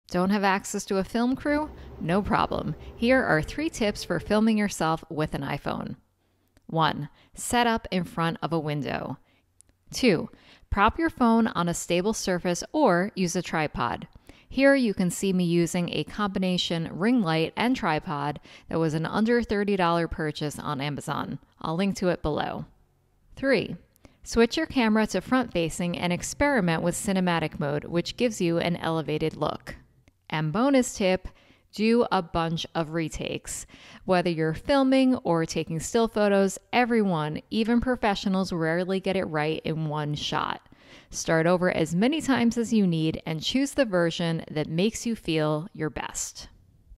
How to film yourself with an iPhone 🤳💁‍♀️🎥 3 Tips
How to film yourself for social media when you don't have a film crew. Here are three easy tips, plus a bonus tactic that all the pros use.